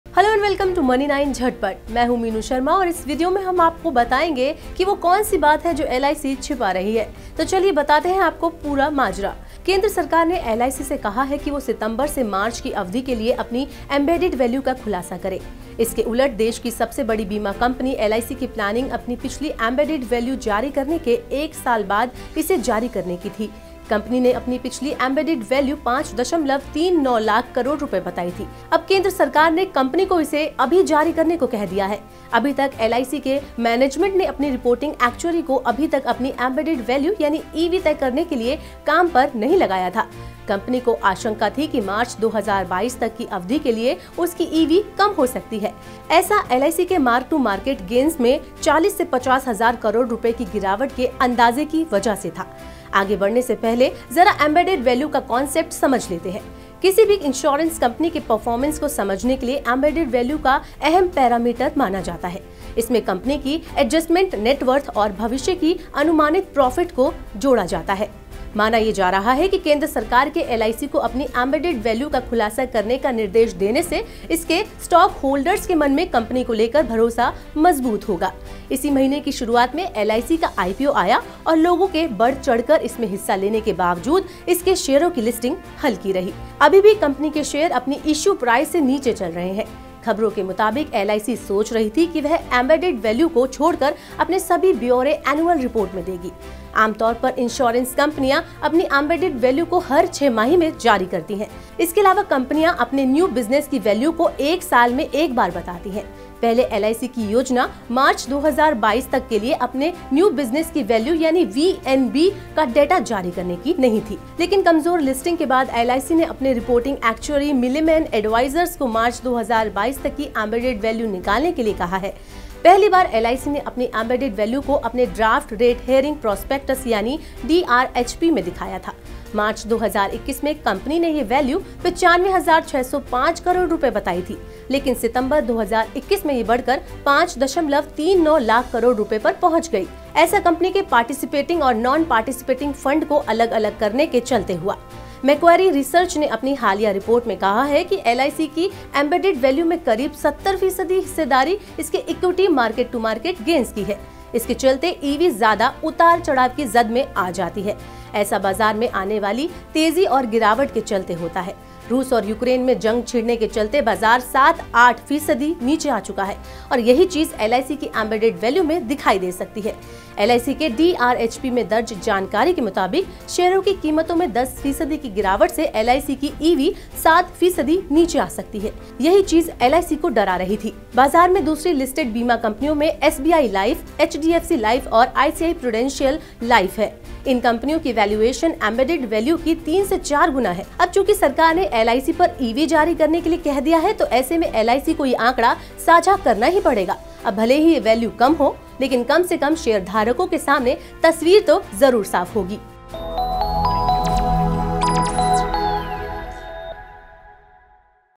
Money9 JhatPat: LIC छिपा रही थी ये बात, Govt ने मांग ली ये खास जानकारी | LIC Embedded Value
केंद्र सरकार (Centre Govt) ने LIC से अपनी एंबेडेड वैल्यू (Embedded Value) बताने के लिए कहा है. ये वैल्यू मार्च 2022 तक के लिए बतानी है. हालांकि, LIC अभी तक इससे बच रही थी. लेकिन, इसकी वजह क्या थी. देखिए ये रिपोर्ट